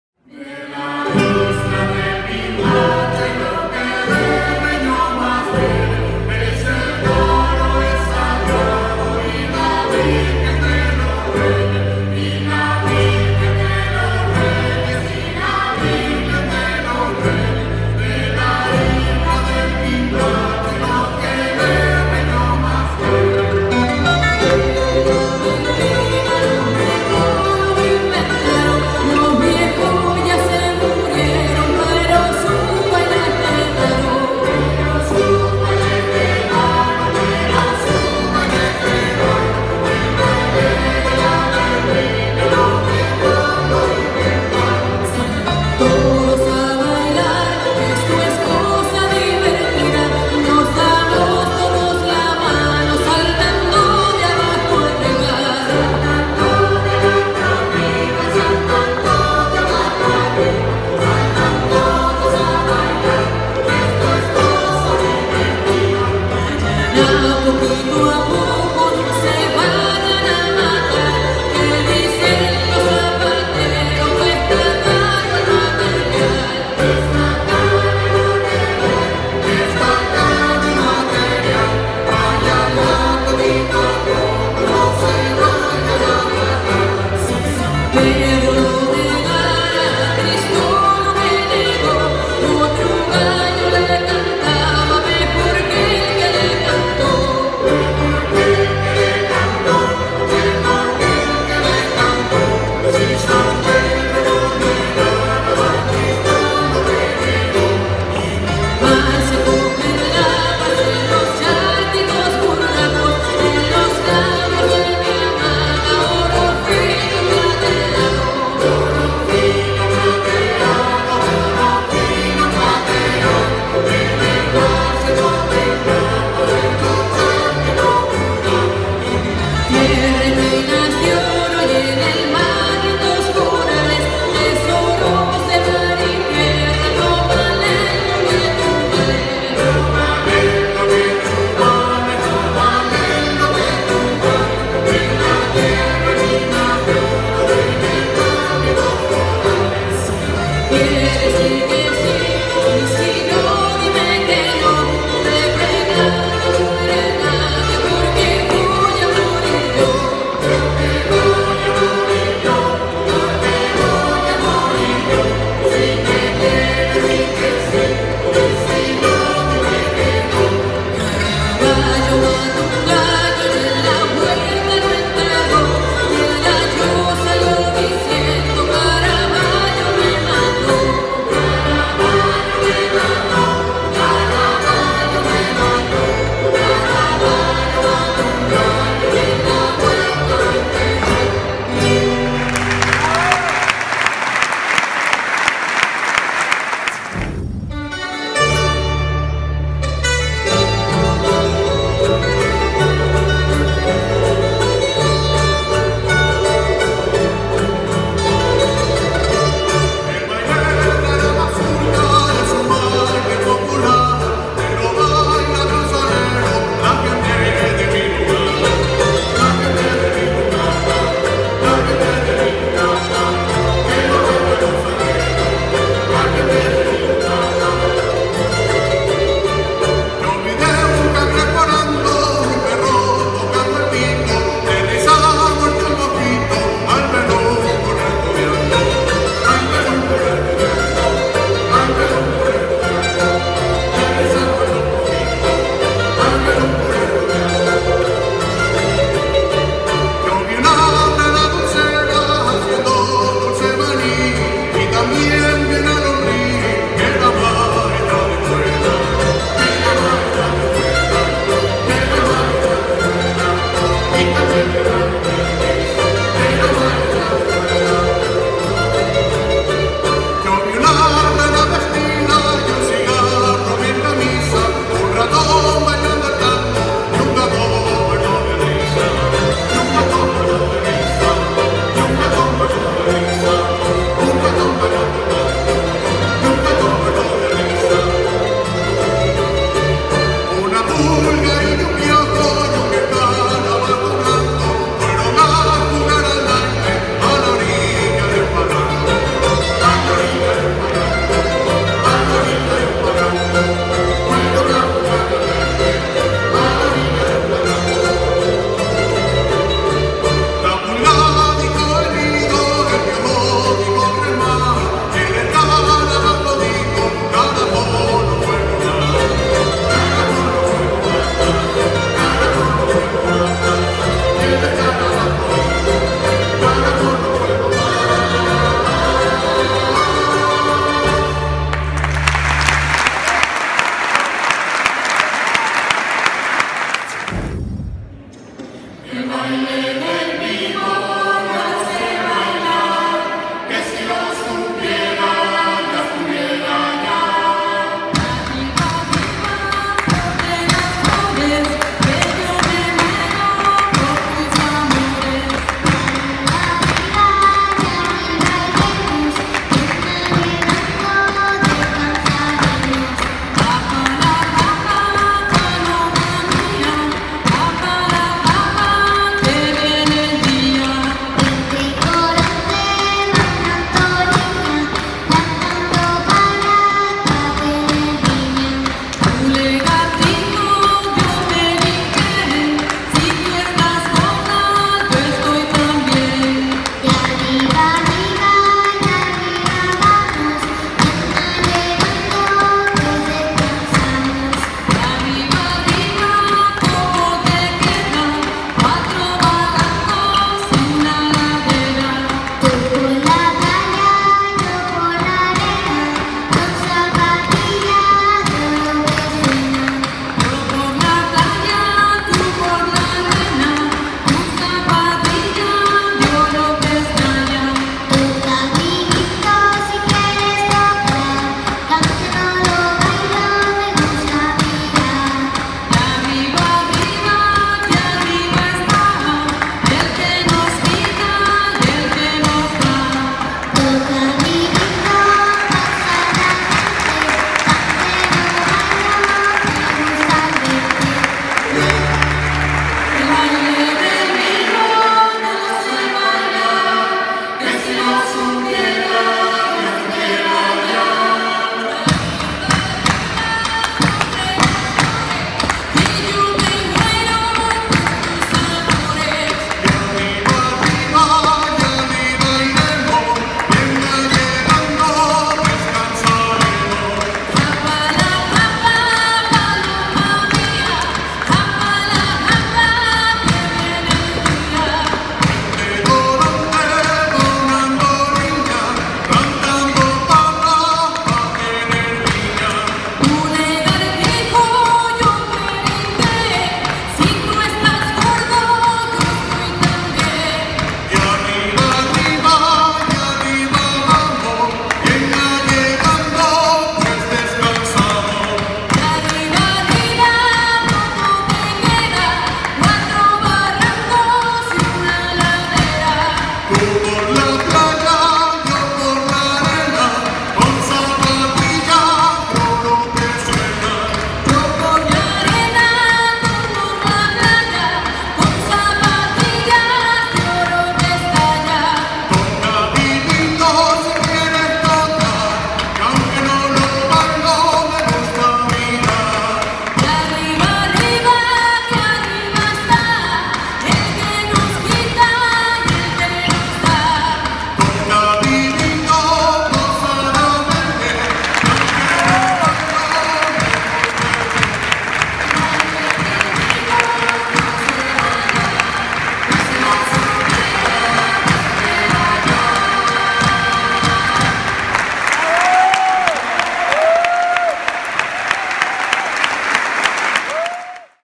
JOAPIRA DEL HIERRO EN EL I FESTIVAL DE MÚSICA Y DANZA TRADICIONAL DE CANARIAS POR LUIS HERNÁNDEZ
Actuación el 19/11/2016  POR LUIS HERNÁNDEZ